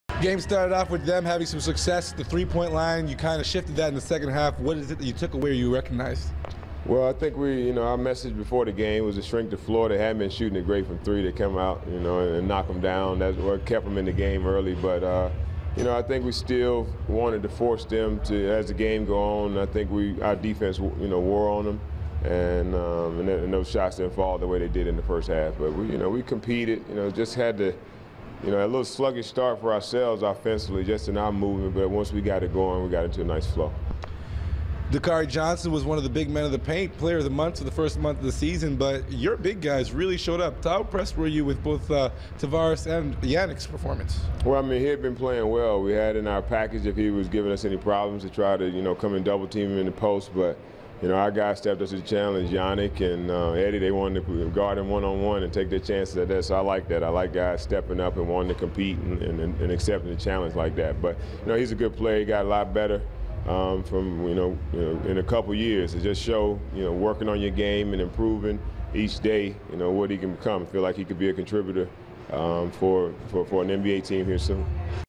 Raptors 905 Post-Game: Jerry Stackhouse - December 15, 2016
Head coach Jerry Stackhouse addresses the media following a 103-91 home win over the Oklahoma City Blue on Thursday night.